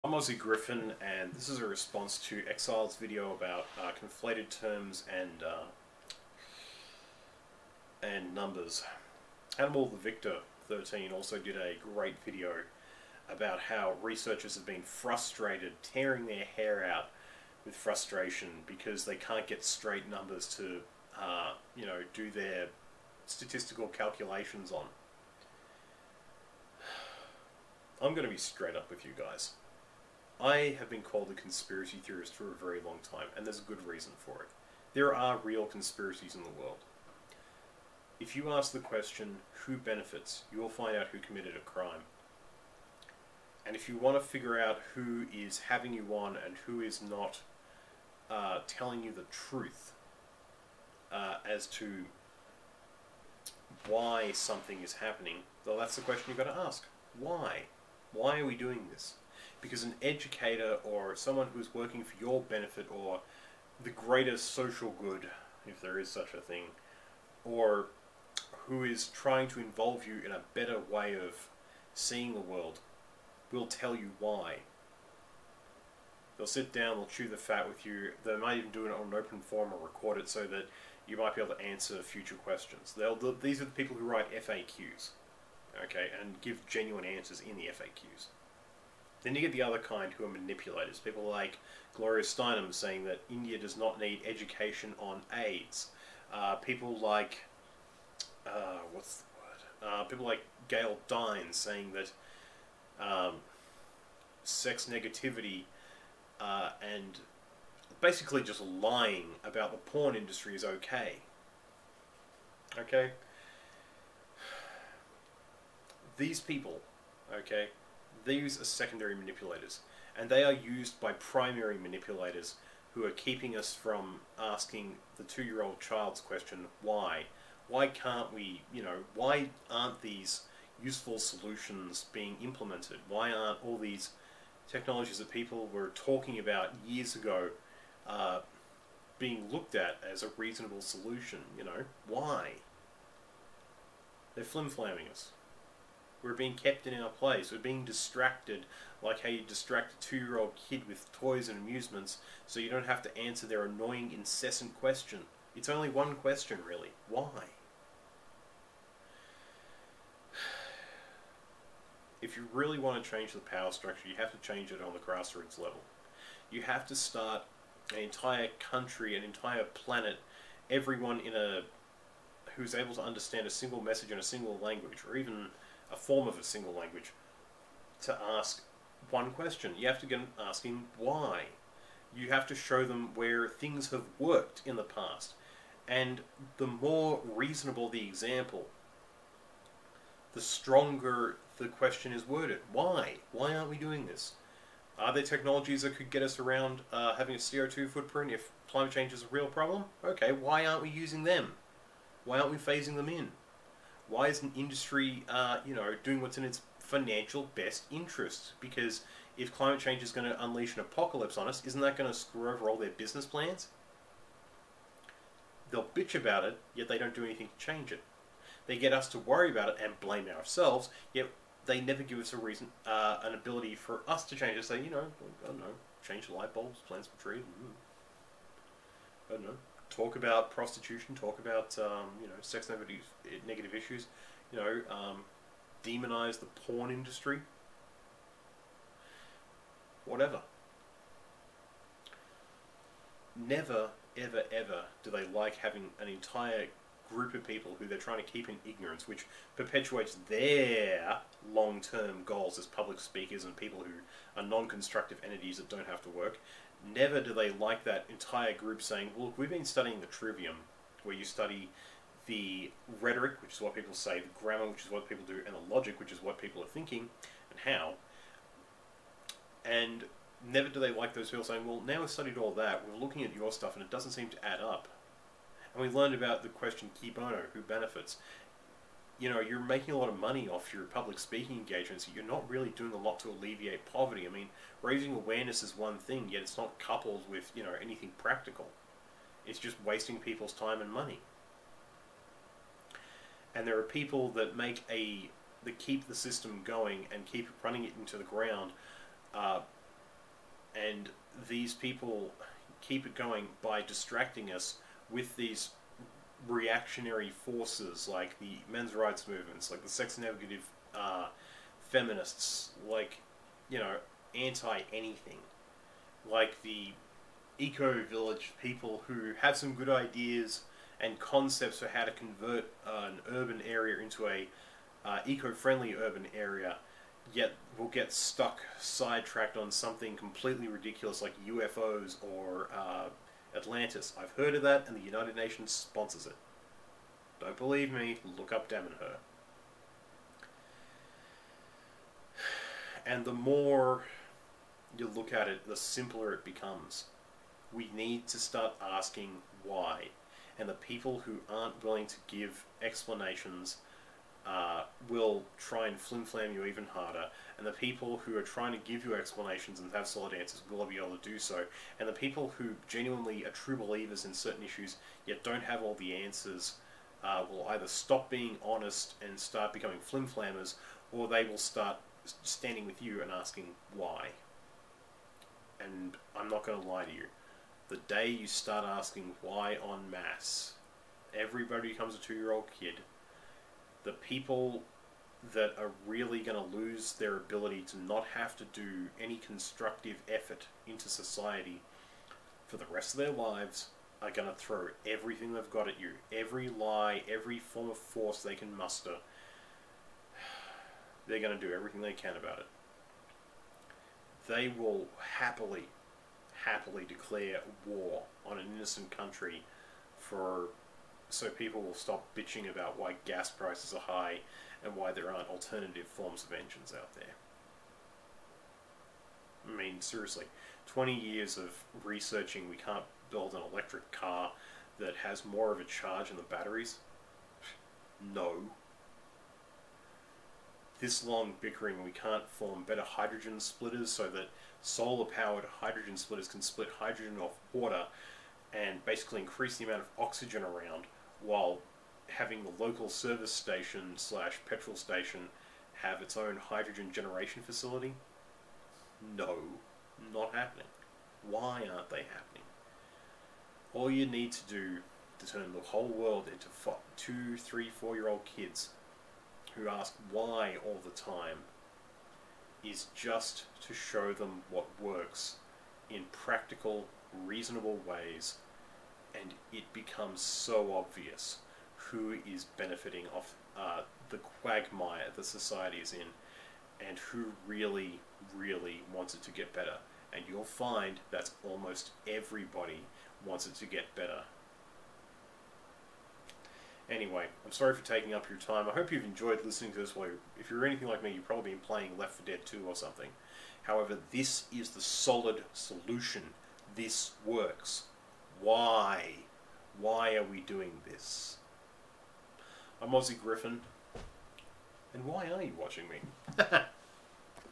Ask WHY to find out who's really trying to help you.
The non-productive entities (Almost counter-productive, except they keep making our problems visible.) have used the reactionary radicals to distract us from what they're doing to society as a whole.
A.G.